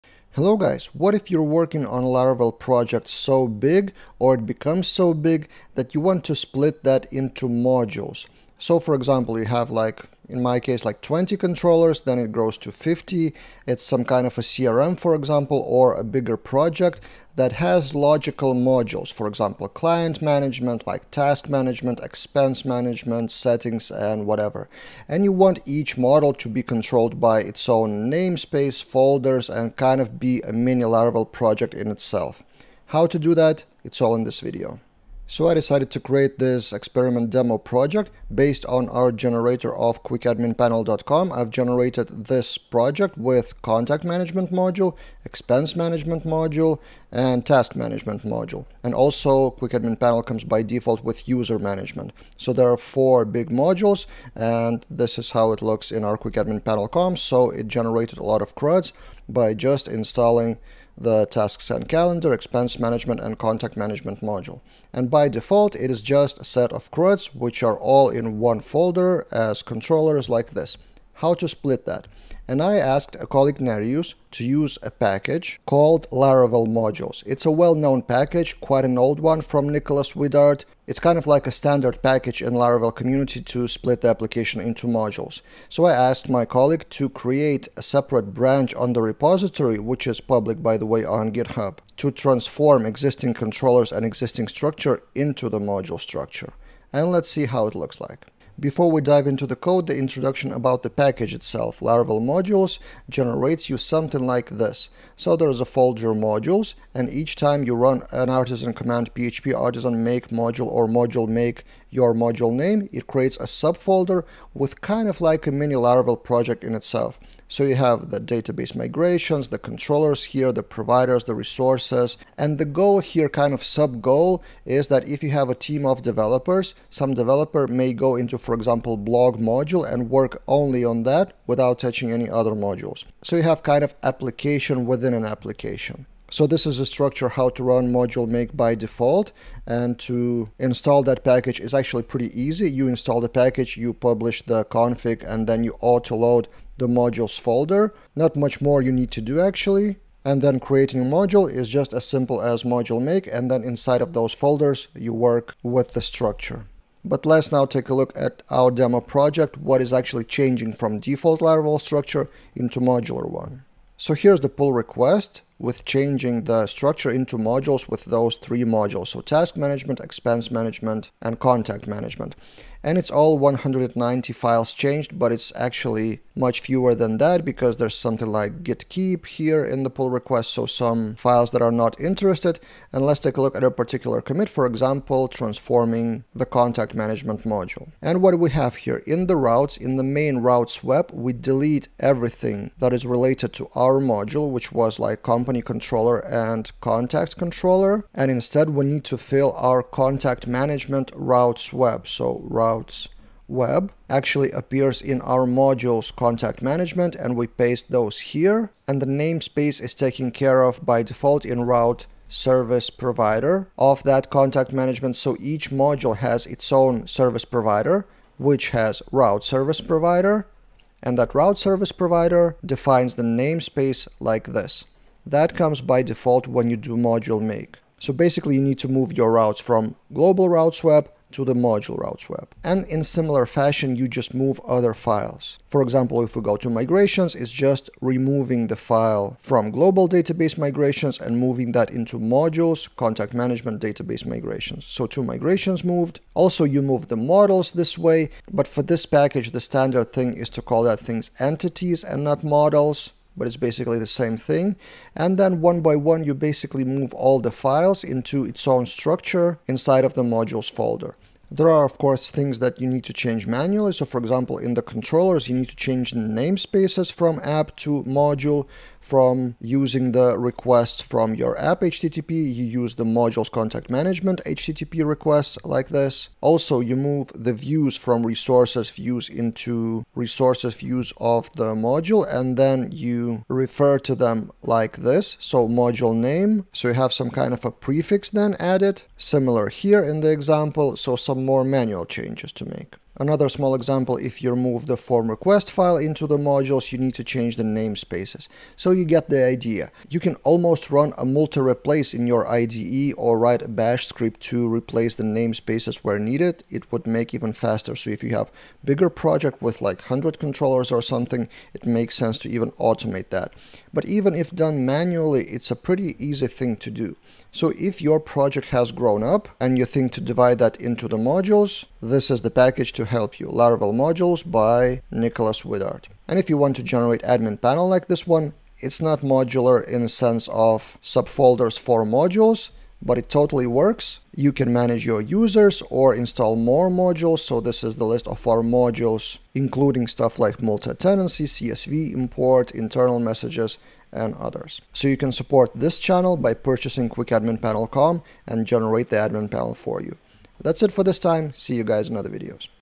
Laravel Modules Demo: Make Your Project Modular
An experiment demo project, transforming a simple Laravel admin panel into a modular structure with the package Laravel Modules.